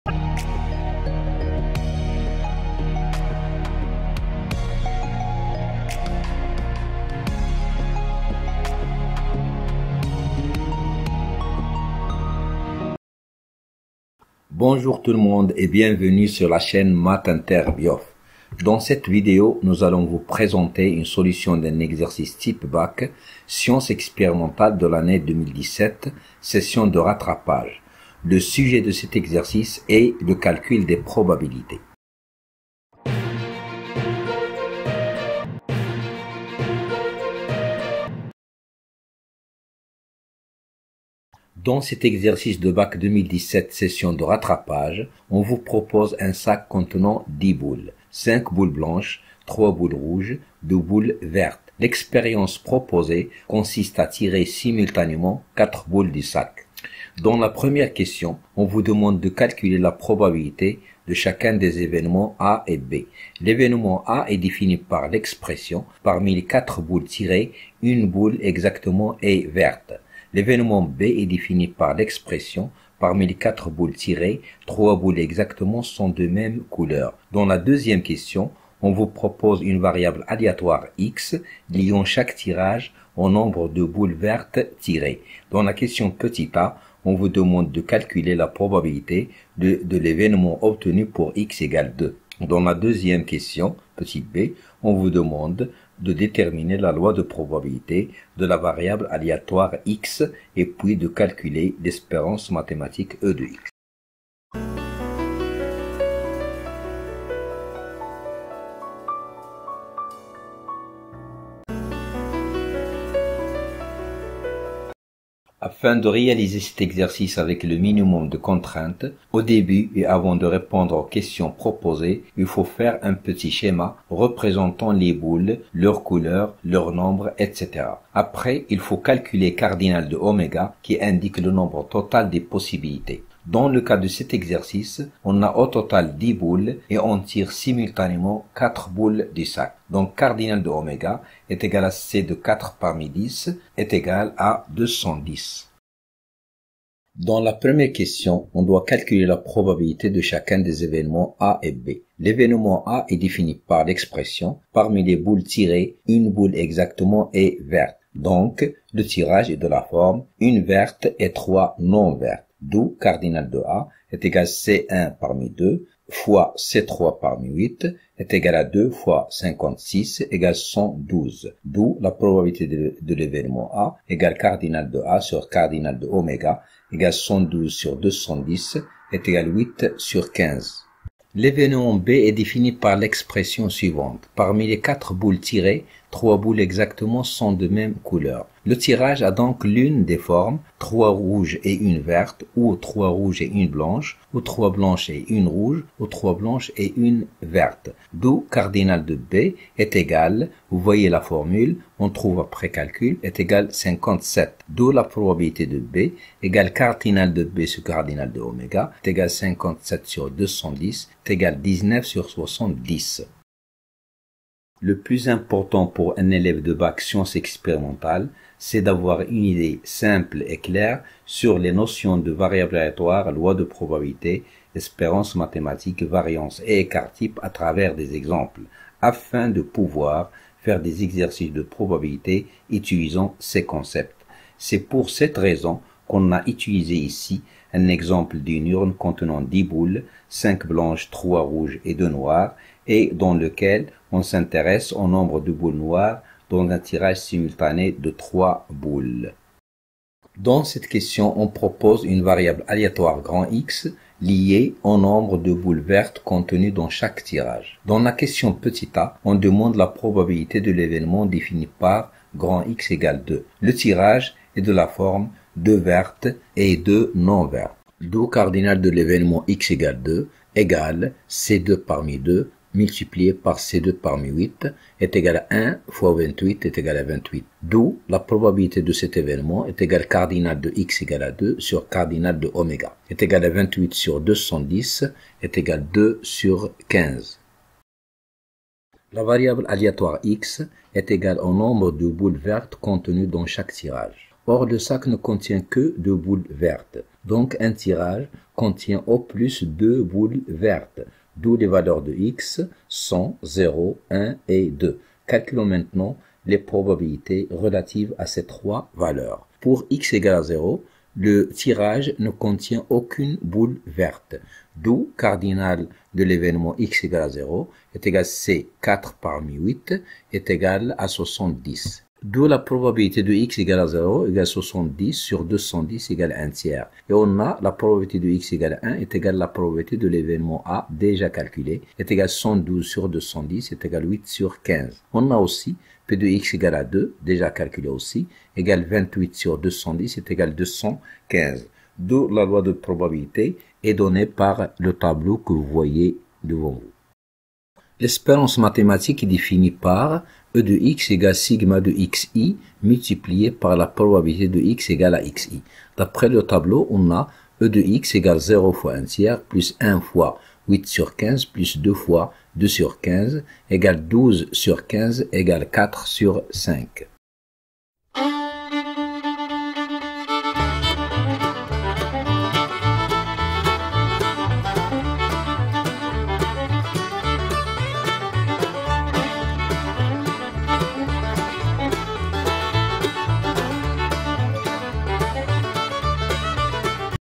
Probabilité solution Bac Sciences Expérimentale Maroc 2017 session de Rattrapage
Cette vidéo présente une solution  d’un exercice type bac (Maroc 2017 Session de Rattrapage)
La solution de chaque question est précédée d’un rappel concernant la question de plus la vidéo est animée d’une musique douce, qui peut donner à l’élève l’envie de faire la recherche avant de de voir la solution qu’on lui propose dans la vidéo